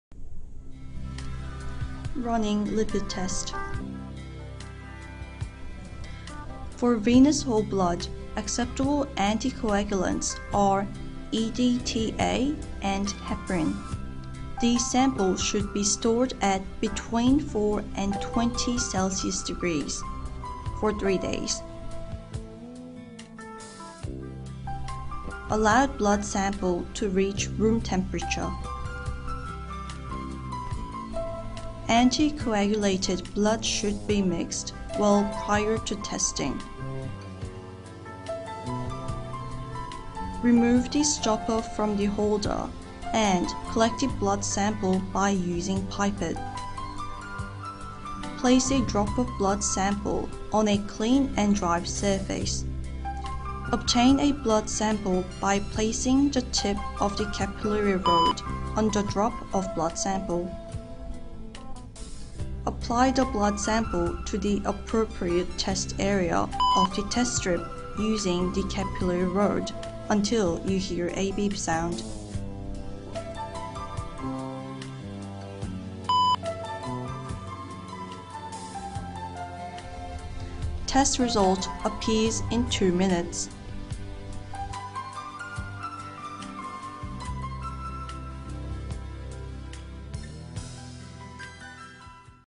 [infopia] LipidPro_ instruction video_#7. Running Lipid test with venous whole blood
[인포피아] 리피드프로_ 사용법동영상_#7. 테스트방법(정맥혈)